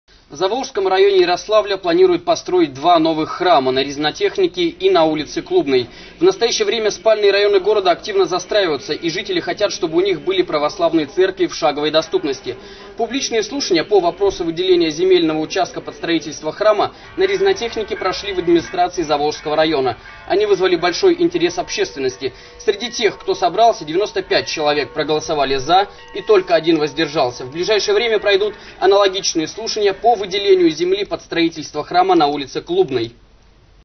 На окраинах Ярославля решено строить церкви в шаговой доступности
В Заволжском районе Ярославля планируют построить два новых храма - на Резинотехнике и на улице Клубной. В настоящее время спальные районы города активно застраиваются и жители хотят, чтобы у них были православные церкви в «шаговой доступности». Публичные слушания по вопросу выделения земельного участка под строительство храма на Резинотехнике прошли в администрации Заволжского района.